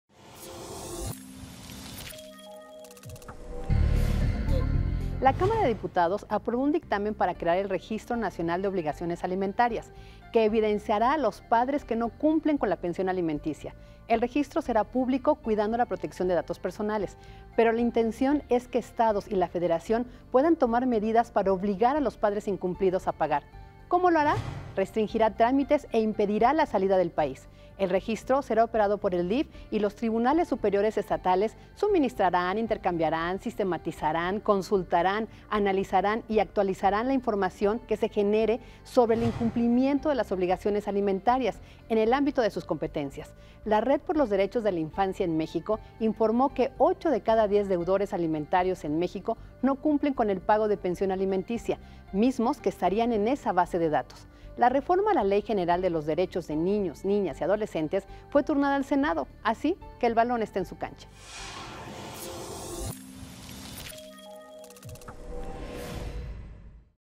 EVIDENCIARÁN A PADRES QUE NO PAGUEN PENSIÓN
La Cámara de Diputados aprobó un dictamen para crear el Registro Nacional de Obligaciones Alimentarias, que evidenciará a los padres que no cumplen con la pensión alimenticia.